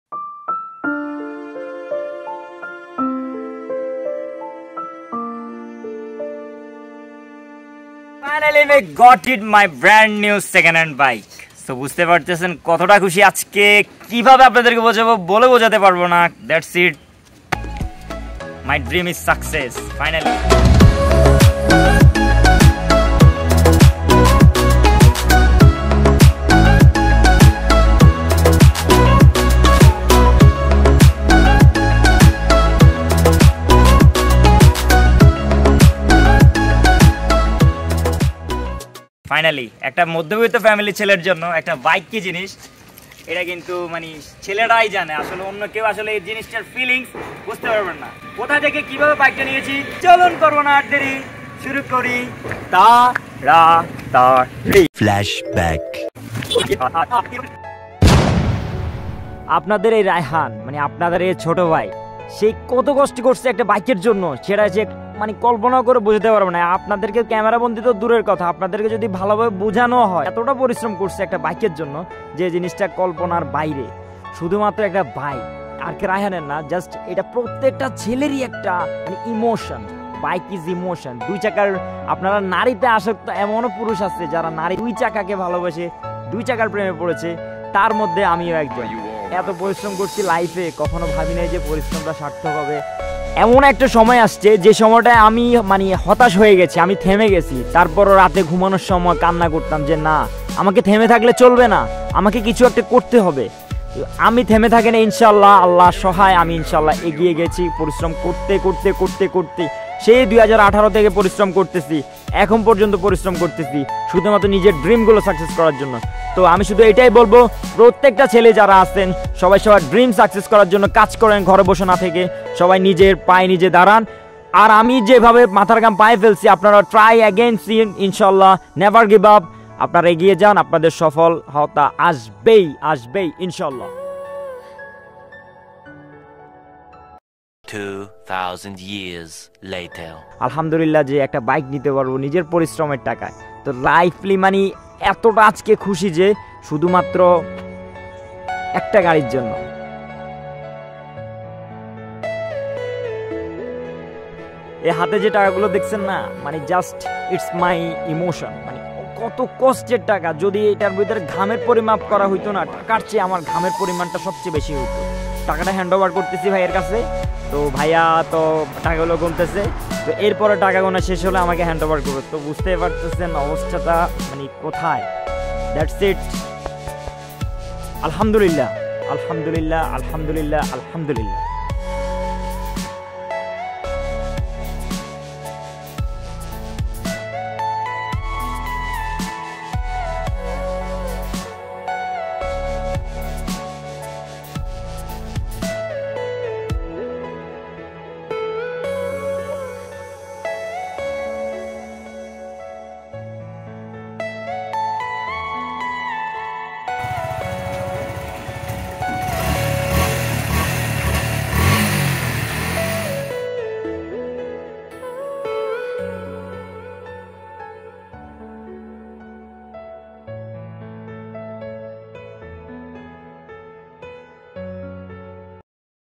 Story by my dream bike🏍️ |Youtube এর টাকায় বাইক কিনে ফেললাম ❤️
আলহামদুলিল্লাহ আল্লাহ তায়ালার কাছে লাখ লাখ শুকরিয়া যে ছোটছোট আশা গুলো পূরণ করতেছে।আমিন